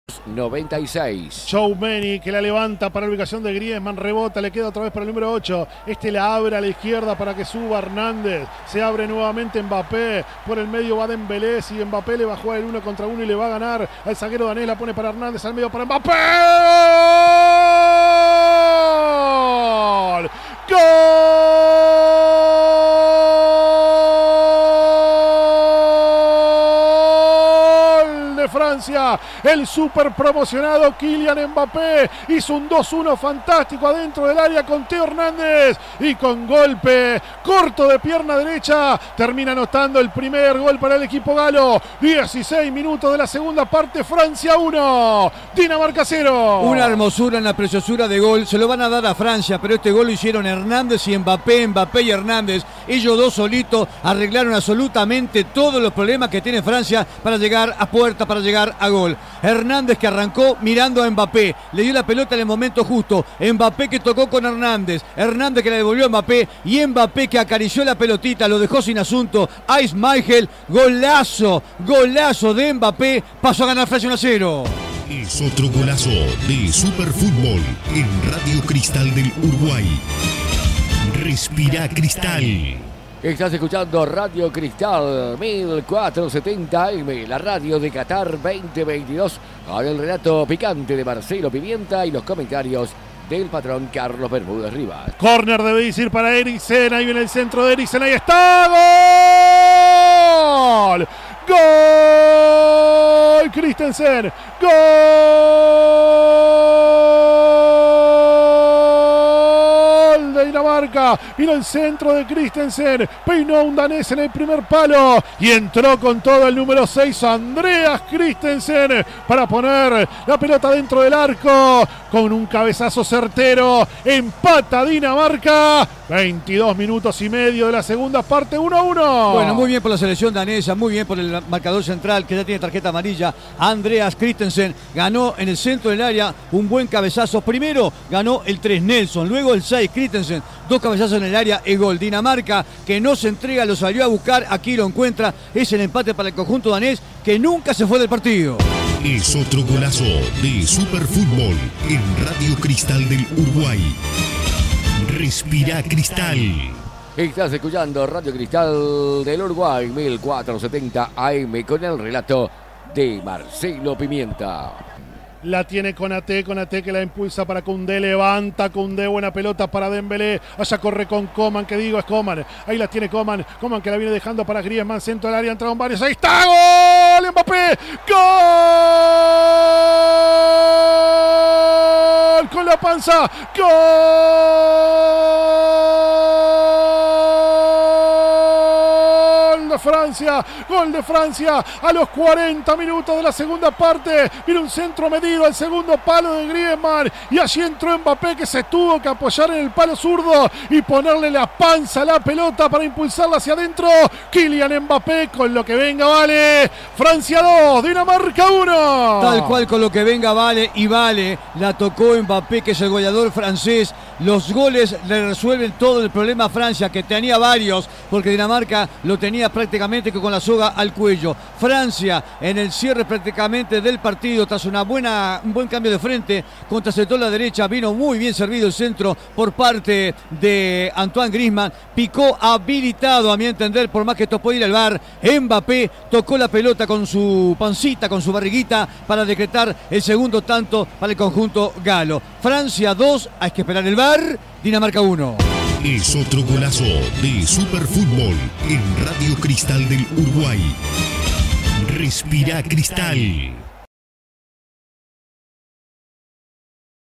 Francia 2 Dinamarca 1 - Copa del Mundo 2022 (por Marcelo Pimienta)
Los goles del partido, por Marcelo Pimienta y Carlos Bermúdez Rivas para Raido Cristal del Uruguay..